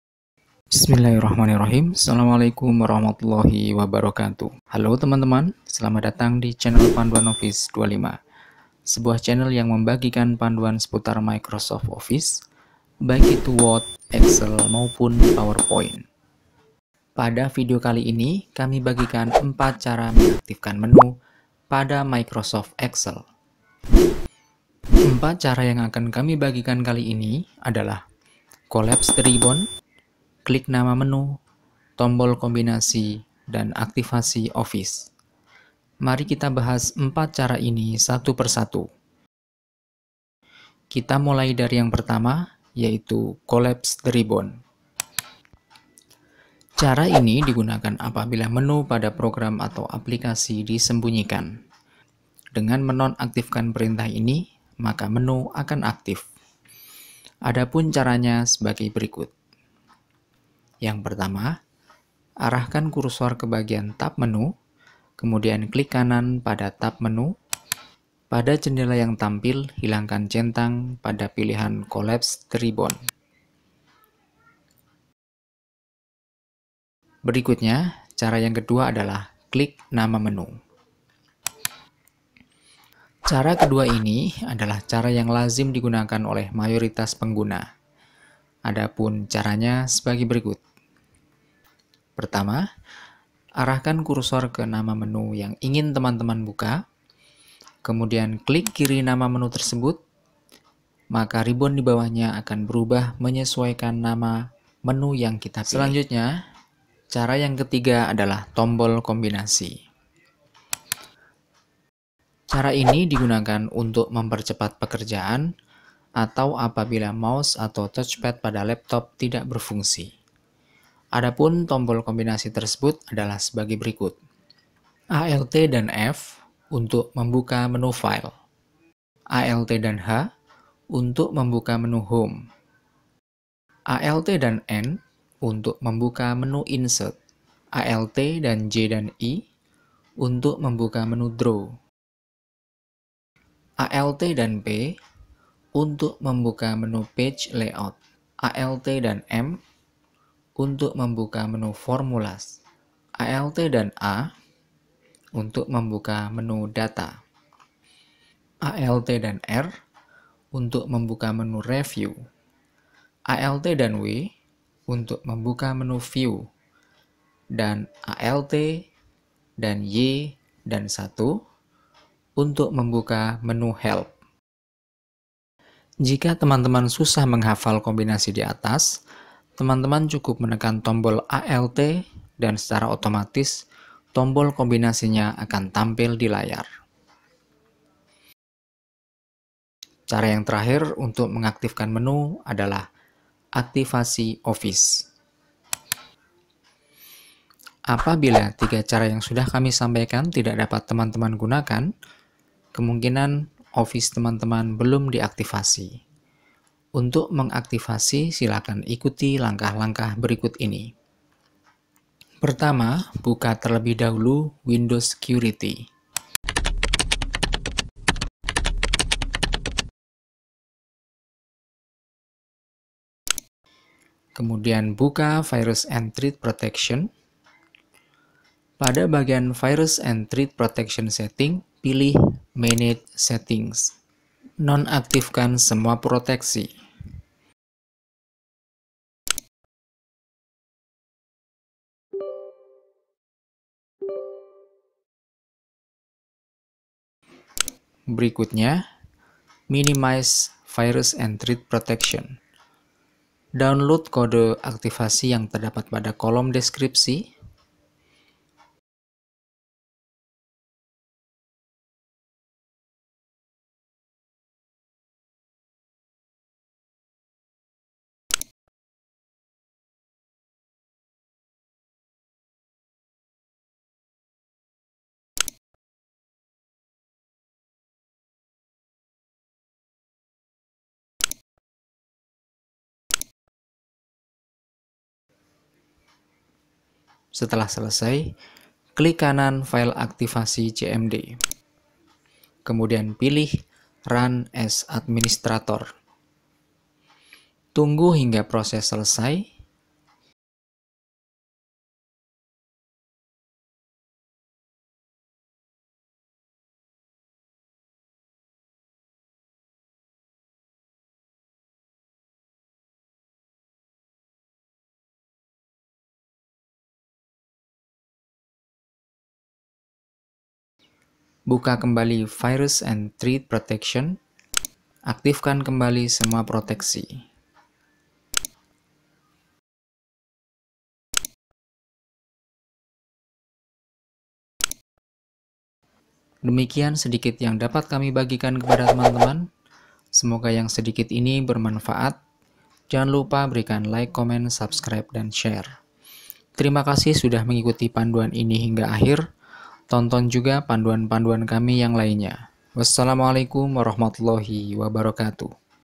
Cara Mengatasi Microsoft Excel tidak bisa mengetik | Tutorial Excel
Cara Mengatasi Microsoft Excel tidak bisa mengetik | Tutorial Excel - Berdasarkan pengalaman sehari-hari, sering sekali orang meminta saya untuk memperbaiki Microsoft Excel pada perangkat mereka, karena tidak bisa diketik.

Itulah alasan saya membuat video ini, untuk saya bagikan kepada mereka yang sering terkendala.

Kendala tersebut biasanya terjadi karena Product Activation Failed / Gagal Aktifasi Produk terhadap Microsoft Office.

apabila menu excel teman-teman tidak tampil atau tidak berfungsi maka bisa menggunakan salah satu dari 4 cara mengaktifkan menu microsoft excel berikut ini. Toton hingga akhir video ini karena ada tehnik aktivasi excel yang di rekomendasikan.

4 cara yang kami pandu di video ini adalah :
1. menonaktifkan collapse the ribbon
2. klik nama menu
3. mengaktifkan menu microsoft excel dengan tombol kombinasi
4. aktivasi office.

*TONTON VIDEO TUTORIAL YANG LAINNYA :*